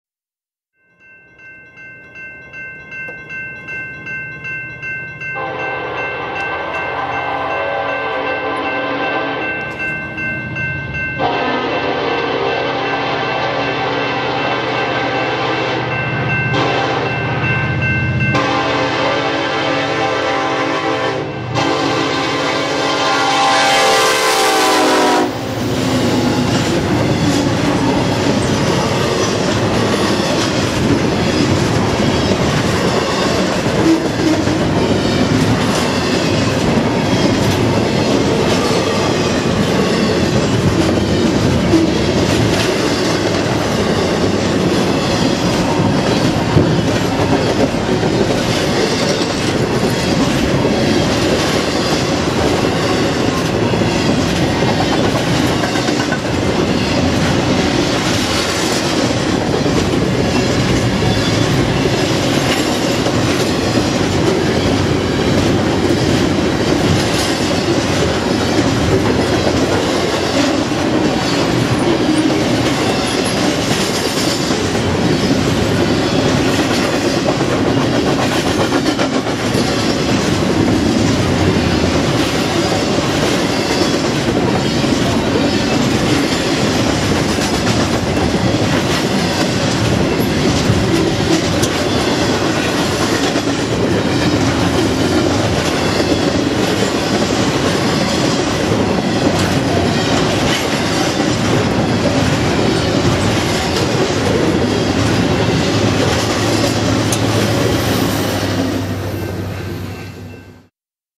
3/11/2015 CNW IN THE FOG UP 1995 Leads The IDUSE Westbound On 1
Yet again worst possible weather for a heritage catch.